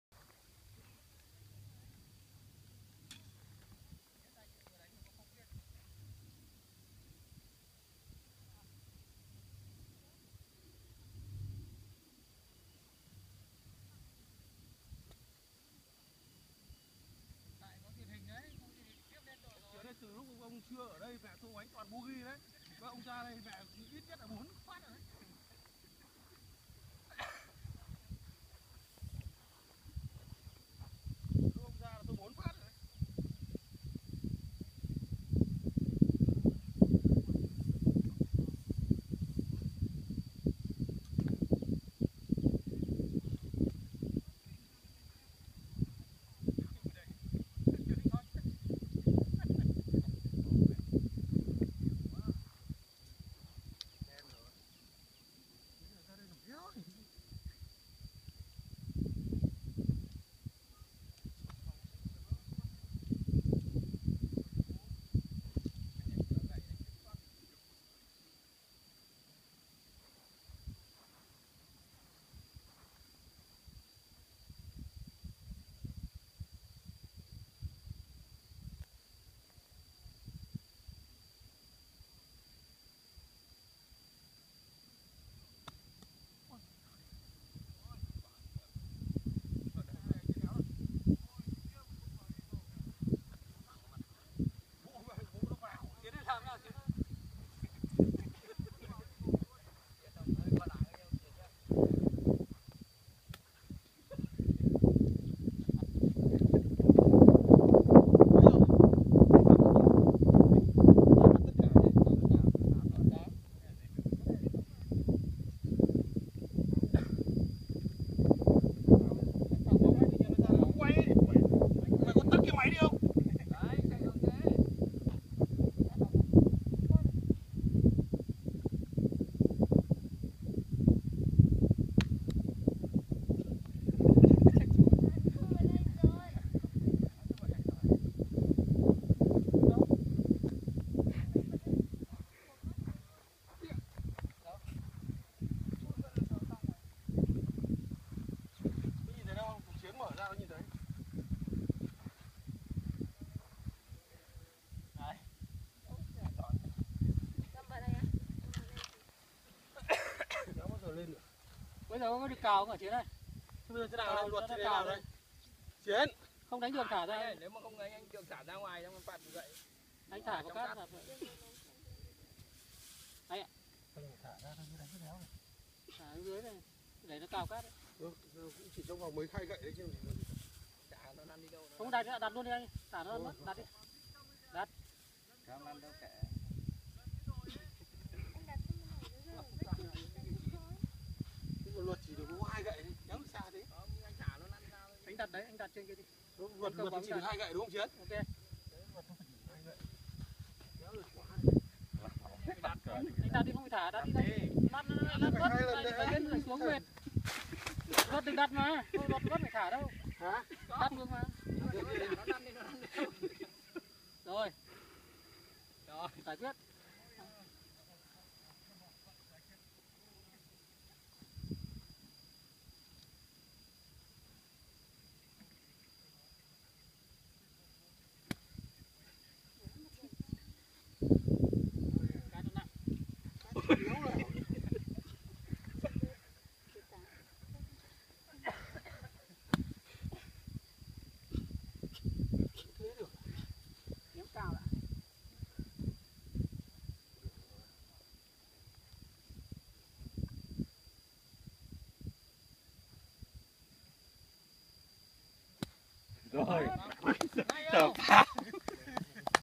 Golfer Vĩnh Râu : par-4 over 15
Golfer Đỗ Văn Vĩnh , đánh 15 gậy tại hố 2C par-4, sân Minh Trí, Sóc Sơn
Trong sự kiện : CASA HABANA GOLF OUTING, ngày 19/6/2015

2.Trang tin tức về golf Việt Nam và thế giới

Tạ Anh Chiến
Admin